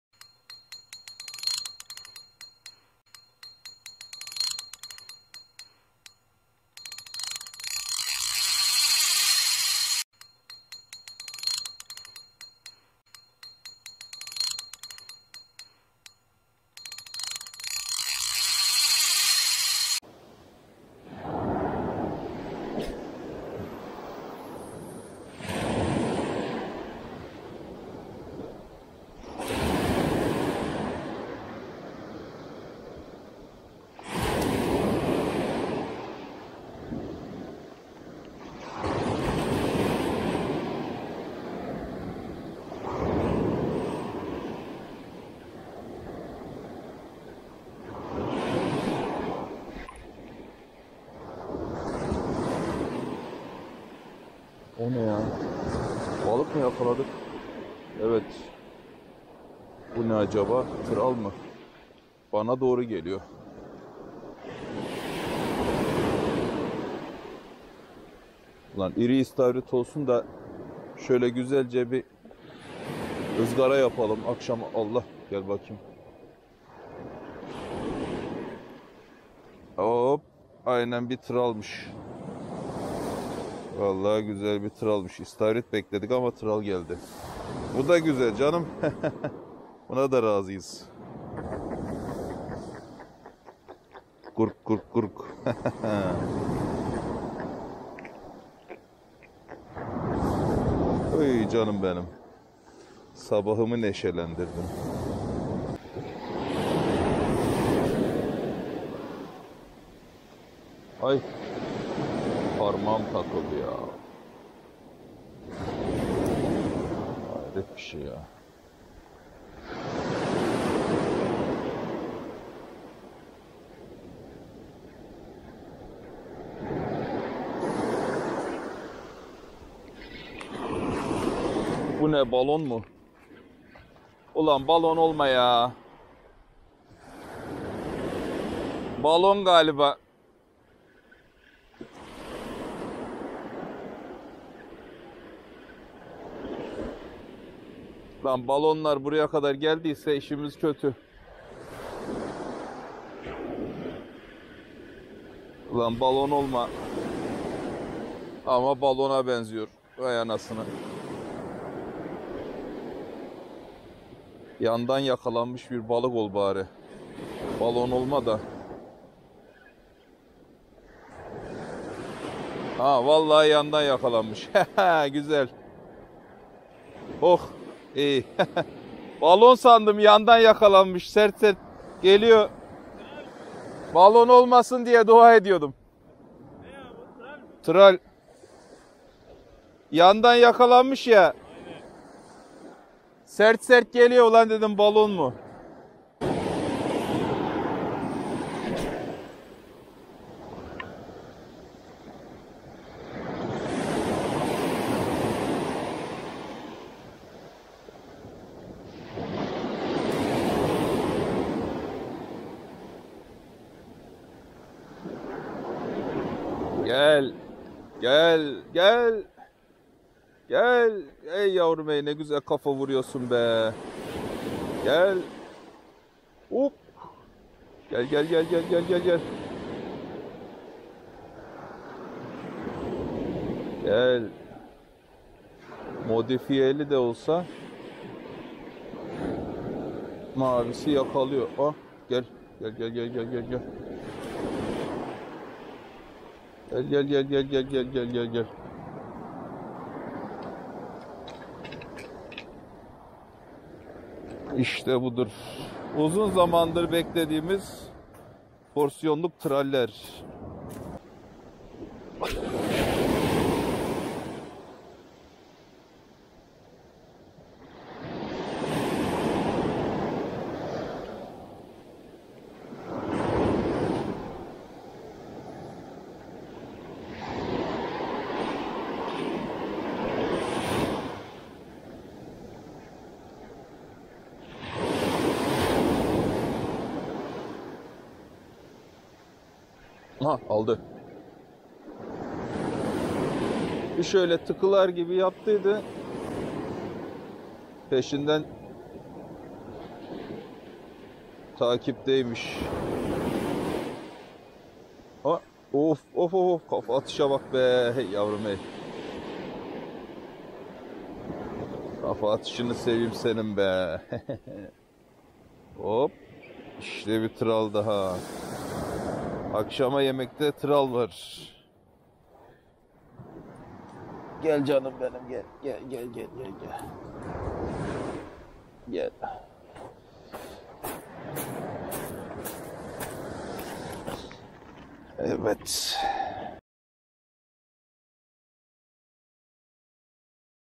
Kıyıdan LRF Mini Jig ile Tral Sportif Balık Avı
Kıyıdan LRF Mini Jig ile Tral Balık Avı nasıl yapılır merak ediyorsan izle.
➡︎ AMATÖR BALIK AVCILIĞINA GÖNÜL VERENLERE SELAM OLSUN ...
🚀 Benimle İletişime Geçebilirsiniz! 🚀
📢 HAKKIMDA 📢
🐦 VİDEONUN BÖLÜMLERİ 🐦